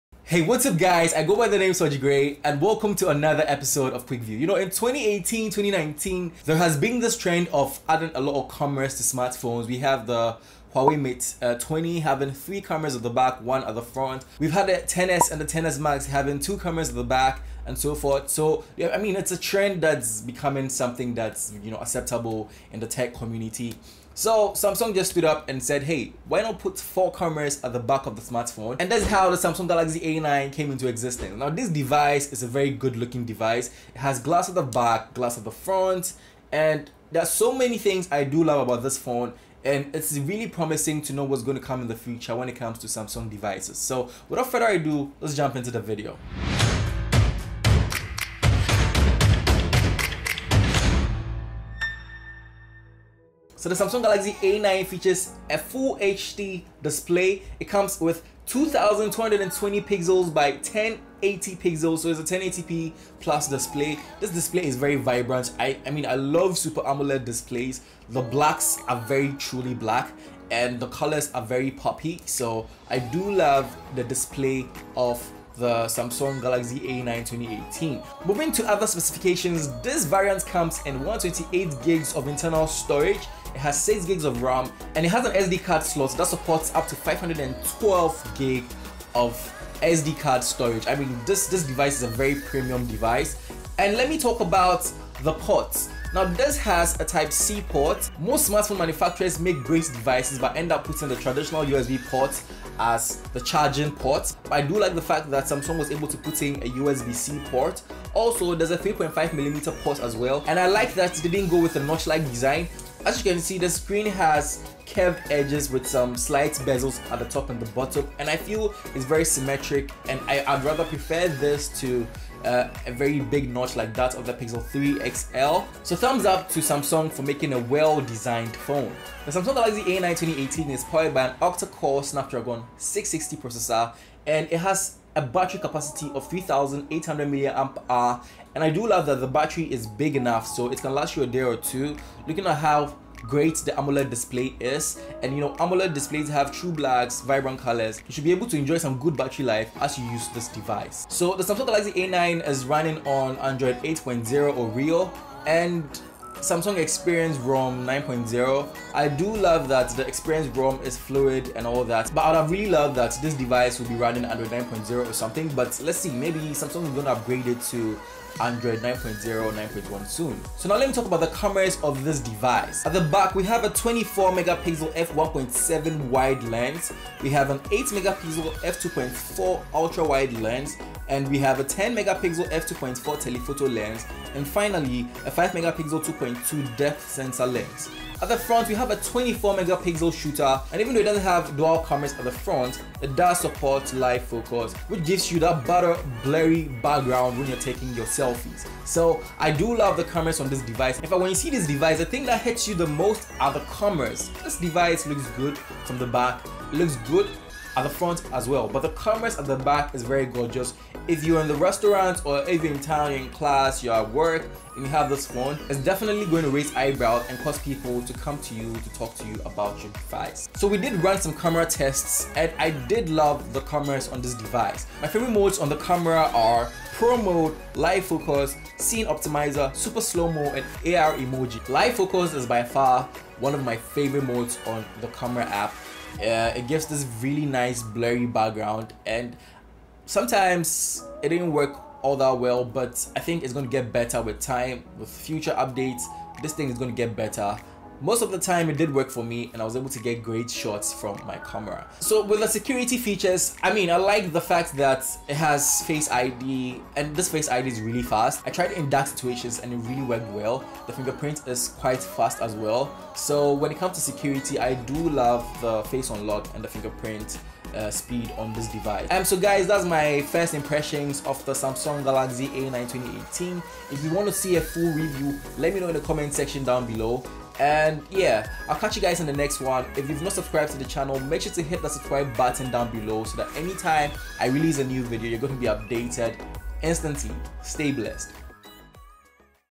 Samsung Galaxy A9 2018 Review: 5 cameras on 1 smartphone!
Reviewed the samsung galaxy A9 2018, a phone with 5 cameras. this phone was made for the selfie lovers and others who will love to up their mobile photography game. leave your thoughts in the comment section, will love to hear from you.